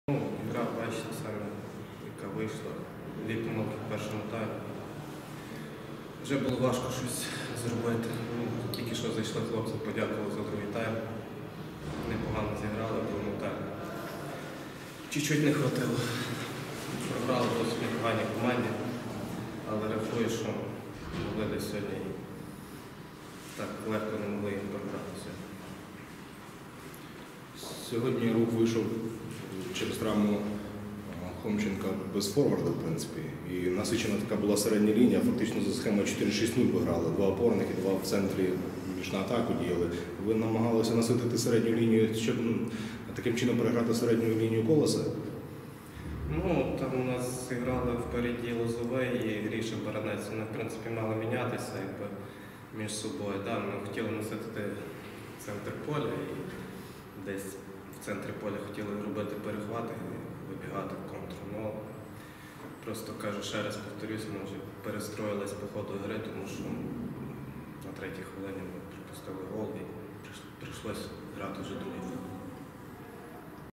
Прес-конференція тренера "Руха" Андрія Кікотя після матчу з "Колосом"
21 жовтня 2017 року